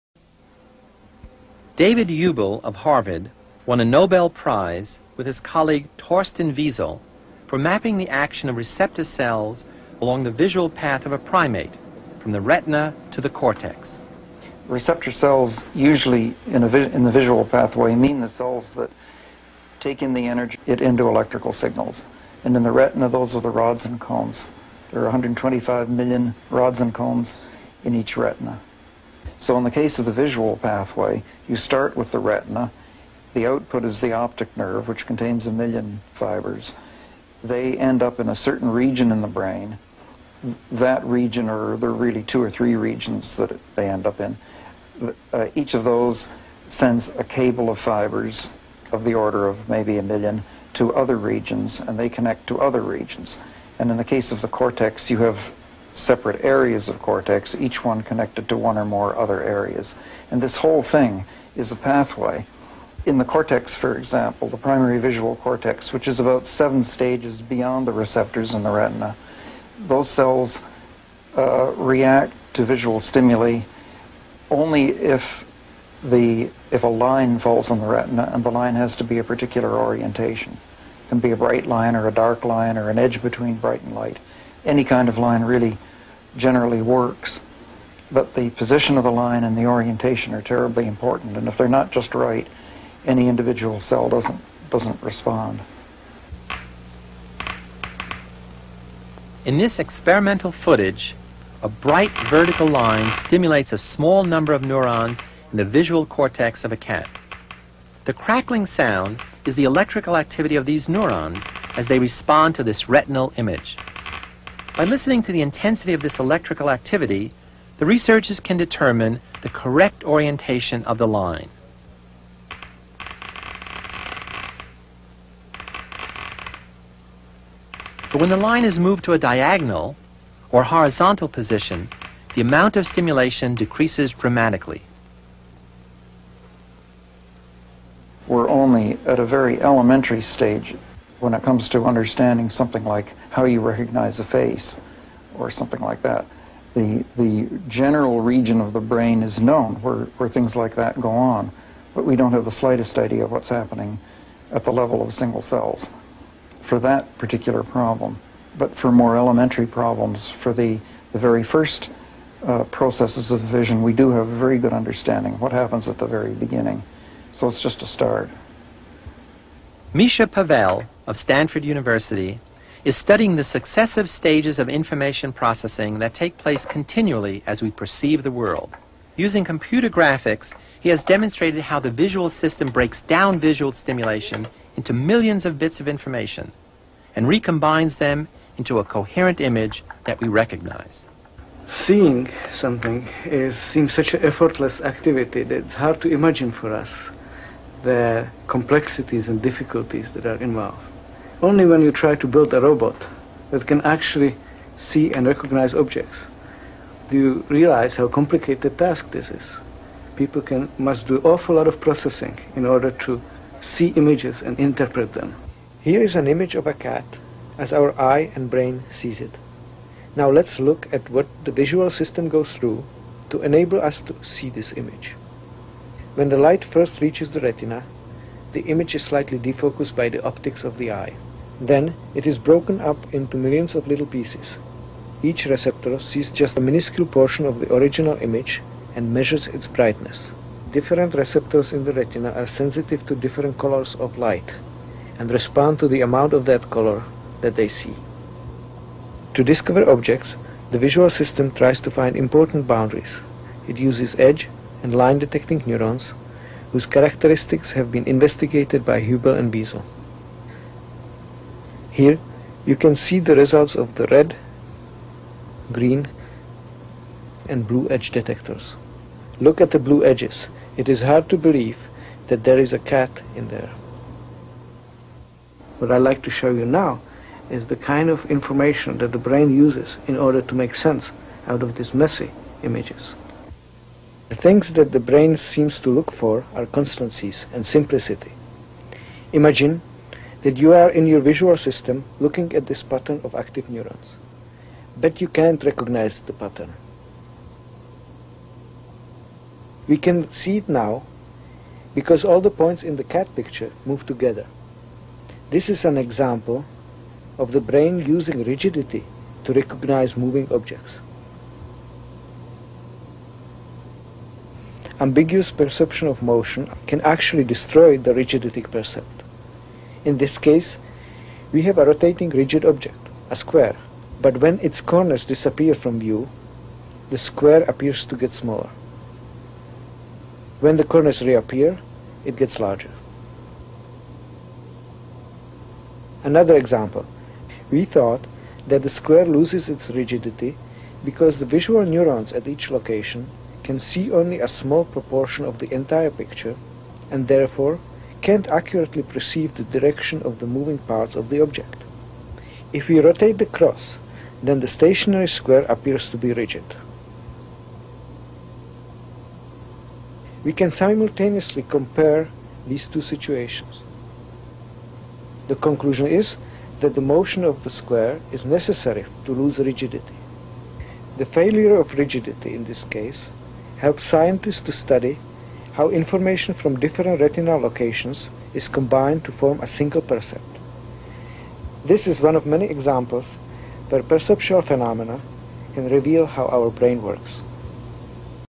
Hubel & Wiesel 1: Intro
Hubel & Wiesel's brreakthrough experiment and discoveries on the how the visual cortex works.